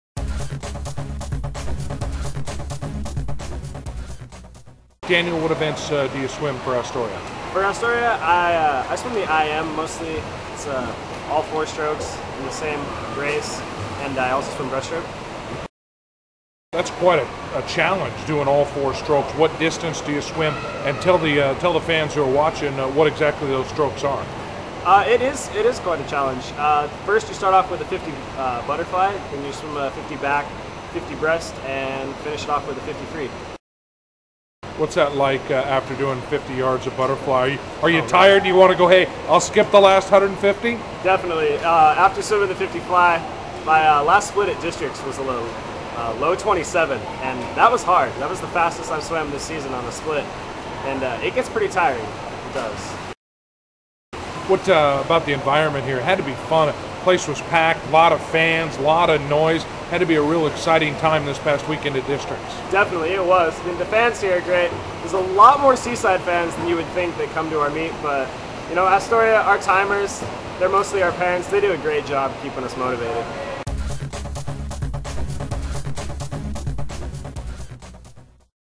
Astoria Ford --- Daniel Perry Interview 02-18
AHS swimmer Daniel Perry talks about the just completed District Swim Meet and the upcoming State Meet. Check out Astoria Ford on Facebook & MySpace. Also signup for Twitter updates to be notified as soon as the next video is posted. AstoriaFord.com --- "You know you want that new Ford"